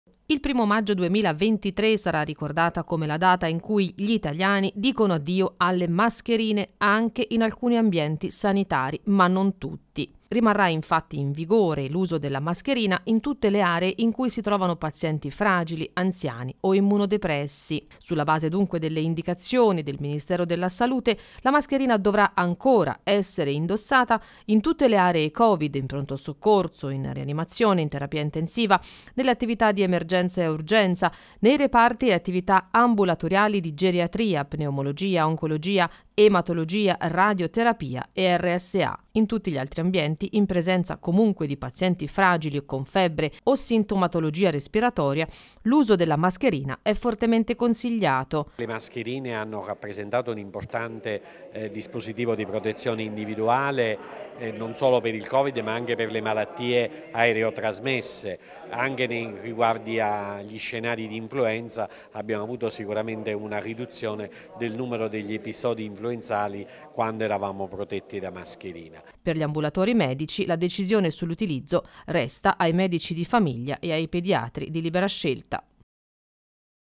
Primo maggio 2023: addio alle mascherine. Ecco dove rimarranno obbligatorie
A partire da oggi, 1 maggio, entrano in vigore le nuove disposizioni sull’obbligo delle mascherine che riguardano lavoratori, pazienti e visitatori nelle strutture sanitarie di tutta Italia. In poche parole, l’obbligo di indossare la mascherina rimane in tutte le aree in cui si trovano pazienti fragili, anziani o immunodepressi.
Sulla base delle indicazioni legislative nazionali, con l’ordinanza del Ministro Schillaci, le Aziende sanitarie hanno individuato le aree all’interno delle strutture sanitarie in cui il dispositivo deve continuare ad essere indossato per motivi di prevenzione e sicurezza.
Pazienti e visitatori dovranno continuare ad indossare la mascherina:
in tutte le aree Covid, in pronto soccorso, rianimazione, terapia intensiva e attività di emergenza urgenza, nei reparti ed attività ambulatoriali di Geriatria, Pneumologia, Oncologia, Ematologia, Radioterapia e nelle Rsa.
In tutti gli altri ambienti in presenza di pazienti fragili, pazienti con febbre o sintomatologia respiratoria, l'uso della mascherina è fortemente consigliato.
Per gli ambulatori medici, la decisione sull’utilizzo resta ai medici di famiglia e pediatri di libera scelta.